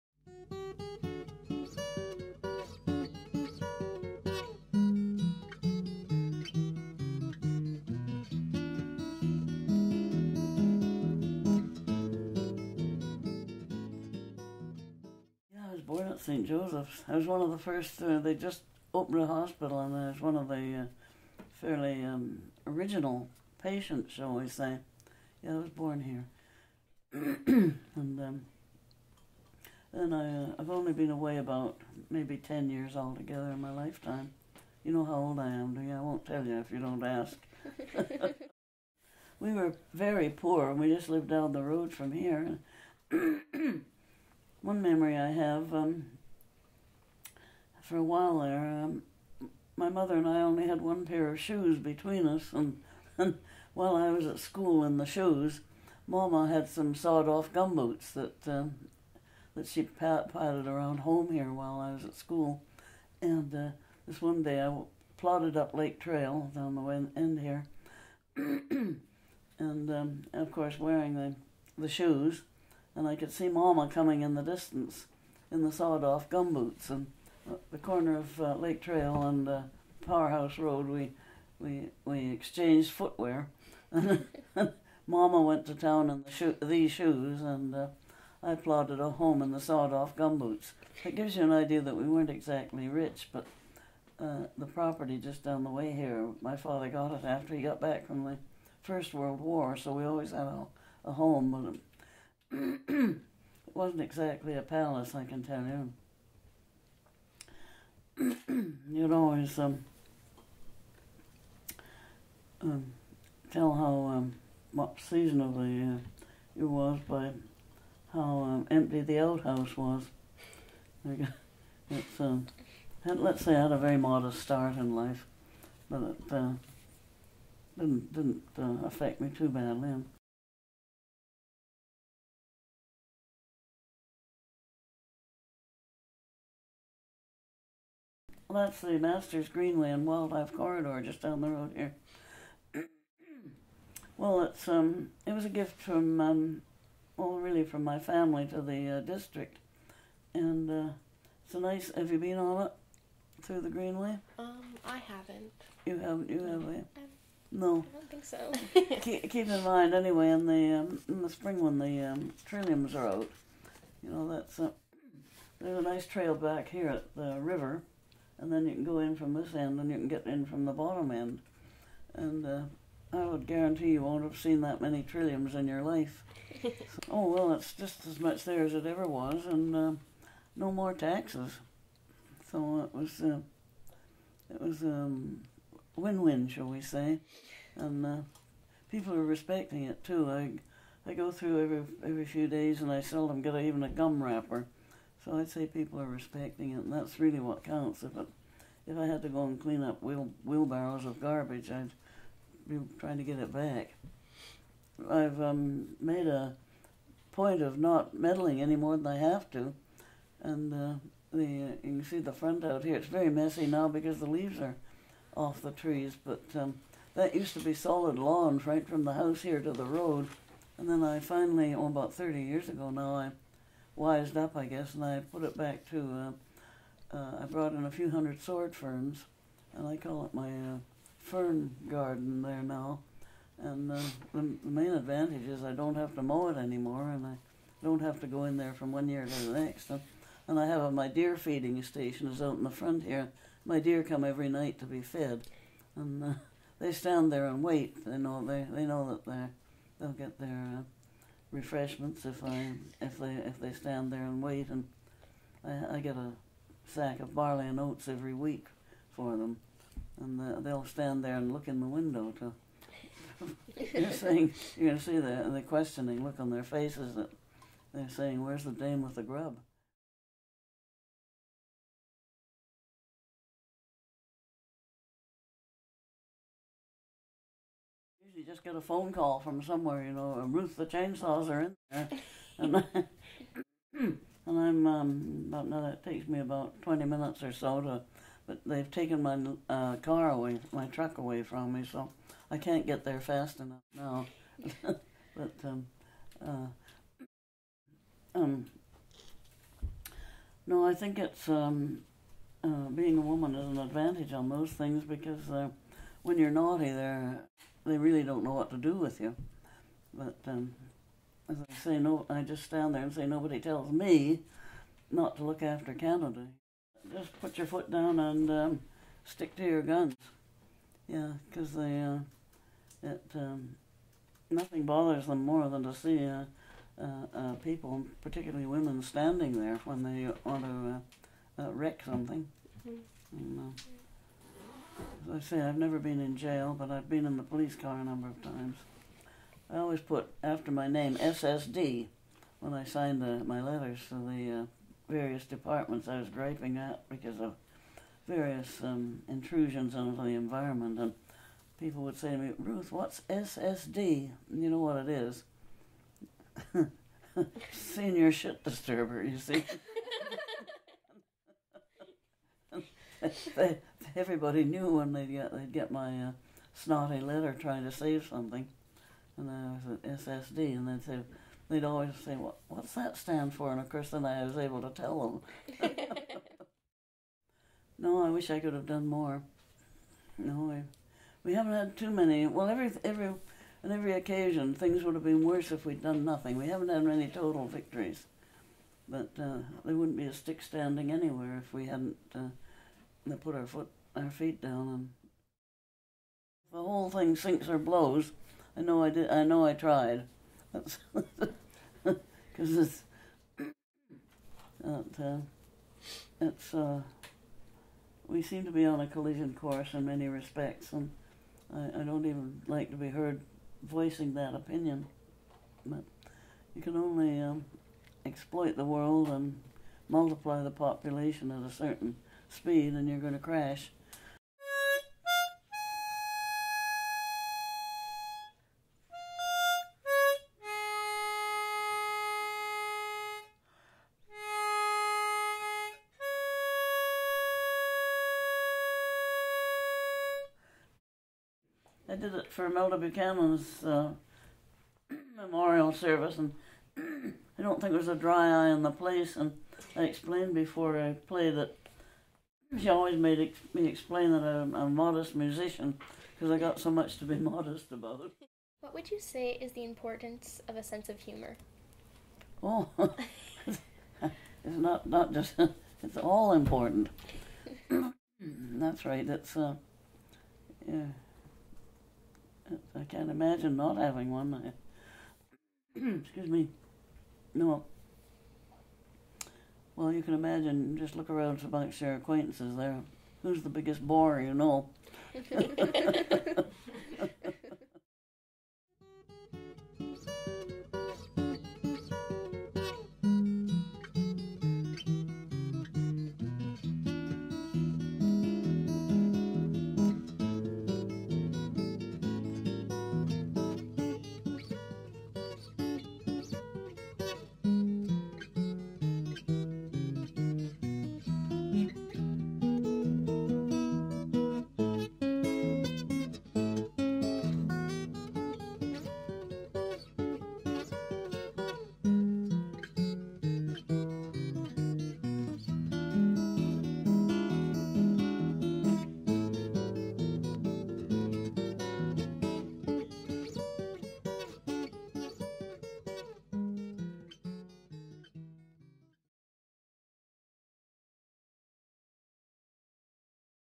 Ruth Masters
Interview for Lake Trail Living History Project, spring 2014.